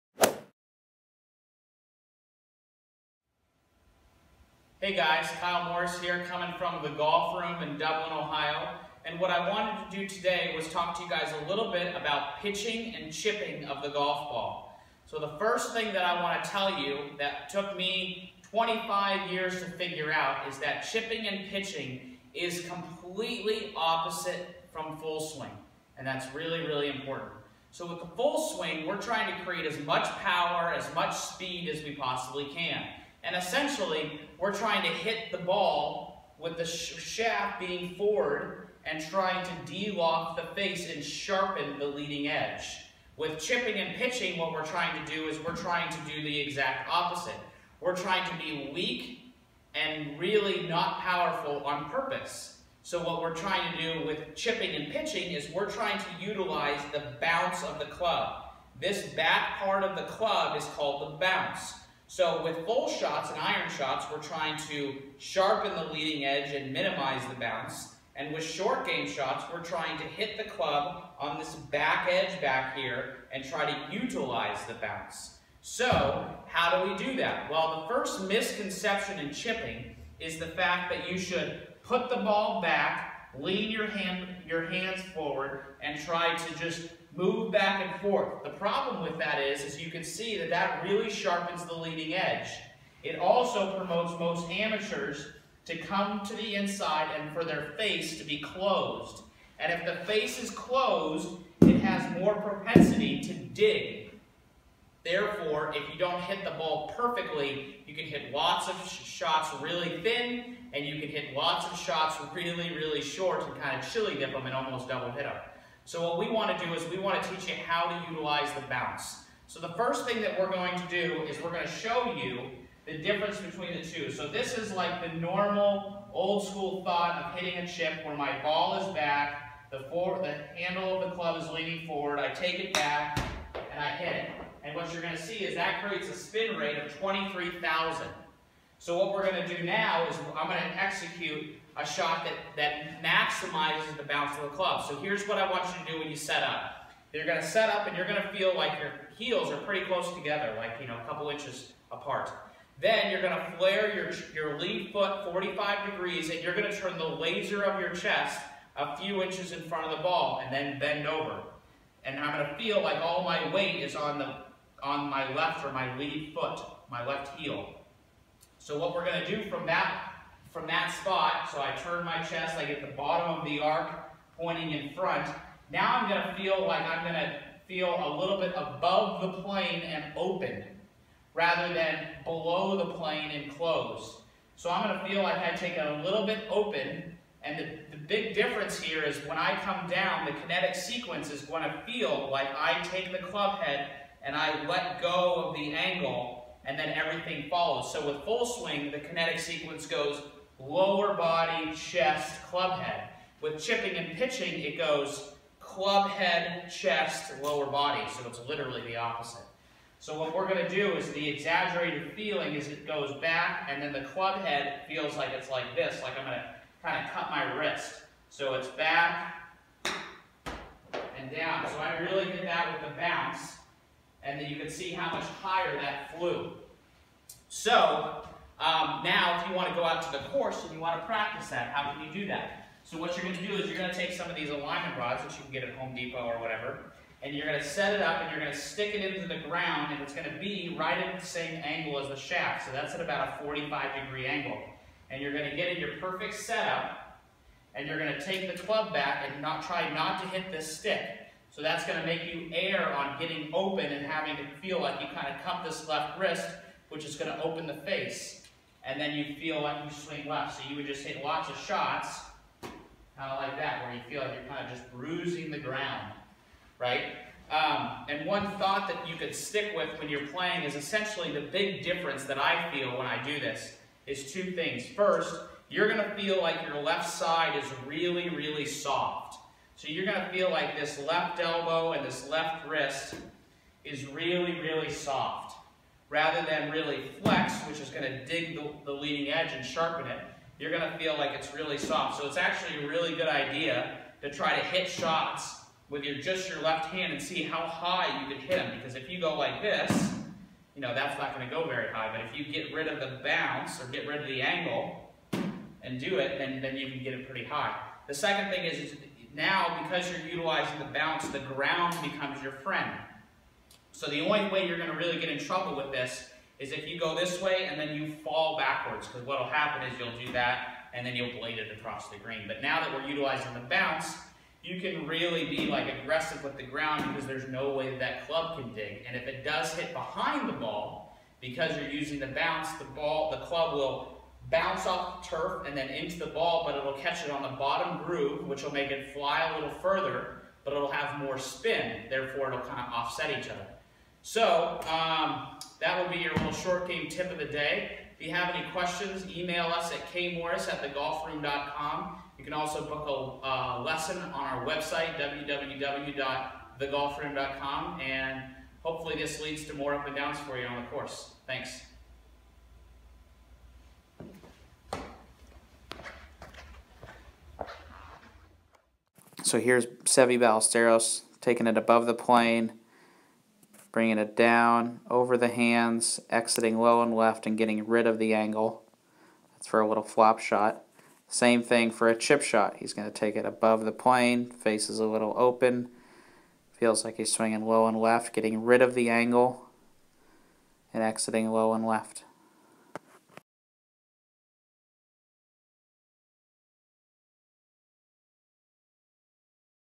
How to utilize the bounce when you chip and pitch
Director of Instruction Kyle Morris show us how to use the bounce when chipping and pitching.